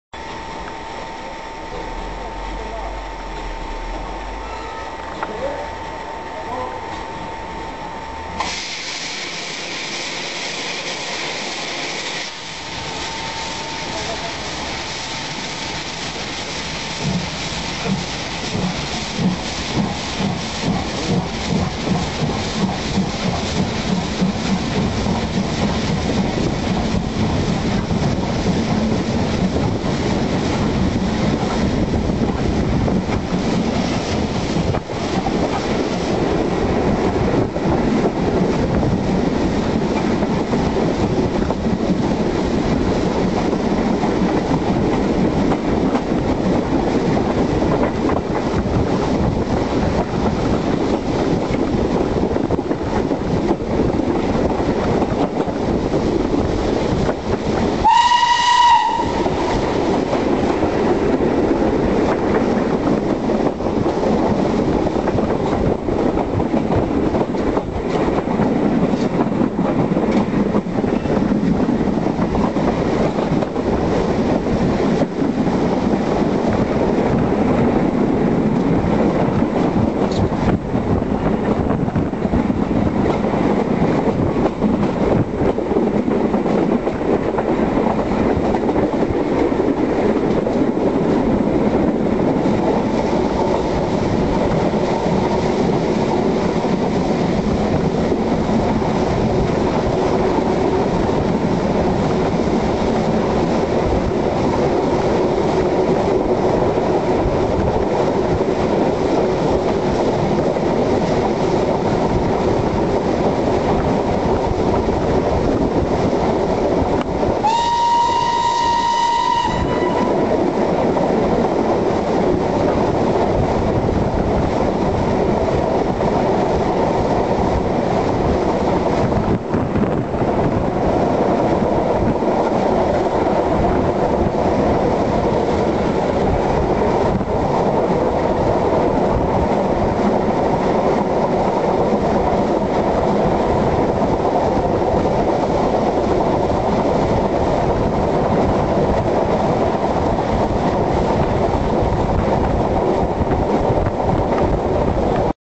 TKt48-18 odjazd z Jelcza Laskowic.
Słabiutka była tekatka tego dnia, pozapychane płomieniówki...